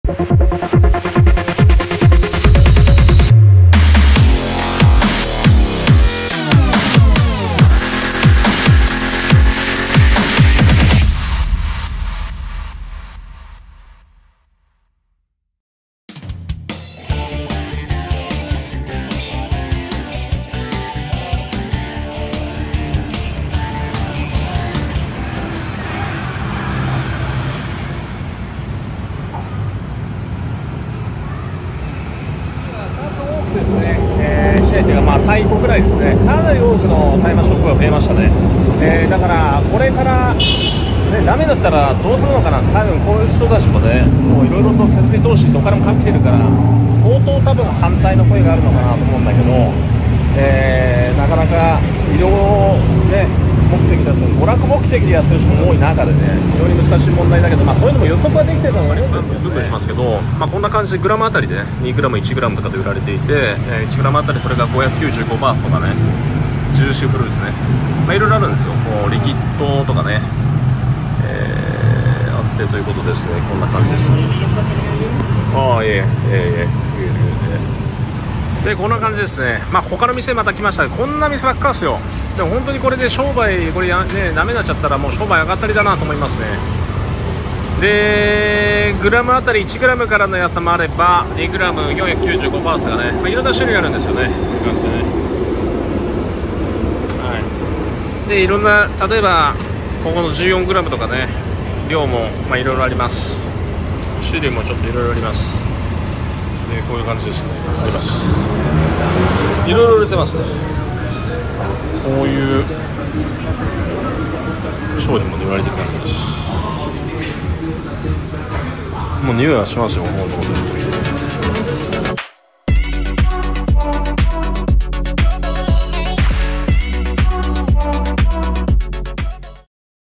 タイのバンコクの大麻(マリファナ)店！大麻合法から娯楽目的禁止に！？規則を厳格化して医療使用のみを認める方針？大麻ビジネス、大麻店にとっては致命的？Bangkok in Thailand
タイは2022年に大麻が解禁されてから、数多くの大麻店が出来ました。国内各地にあり、バンコク市内には至る所にあります。大麻中毒になり、弊害も多く生まれているのを危惧して政府は今年に入り、大麻の娯楽目的禁止、医療用のみの使用にする法案を通そうとしています。

これだけ大麻店が出来ている中、大麻ビジネスの恩恵を受けている人が多い中、もし医療用目的のみになった場合、薬剤師のいない路面店にとつては致命的でしょう。